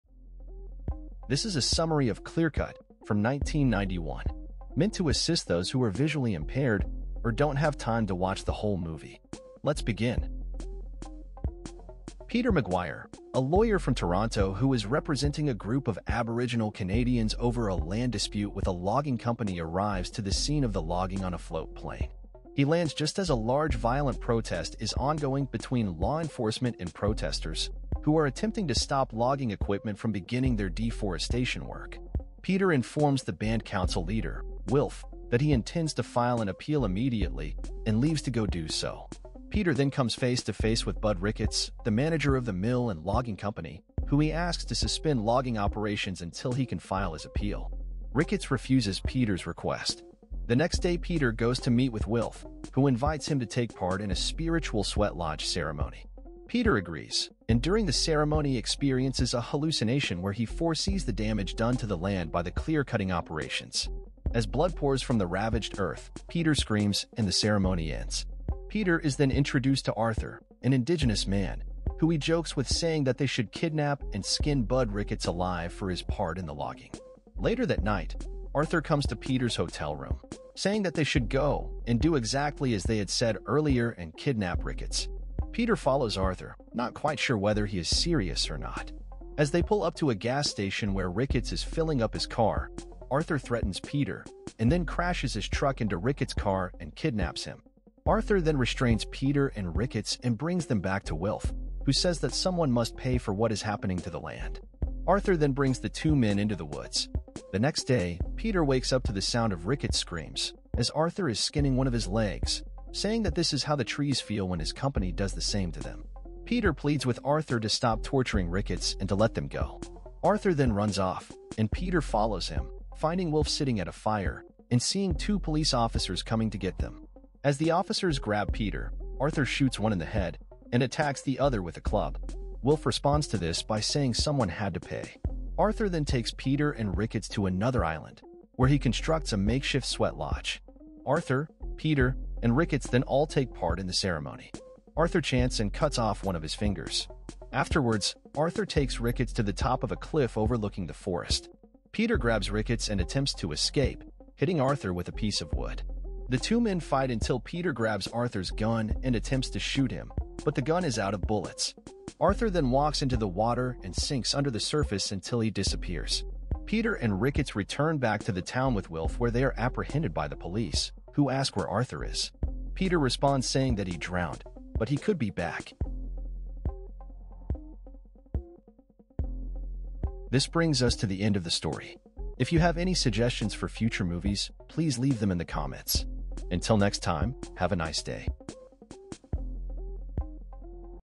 Clearcut (1991) • Movie Recap & Plot Synopsis
An audio synopsis of Clearcut from 1991 meant to assist those who are visually impaired or don't have time to watch the whole movie.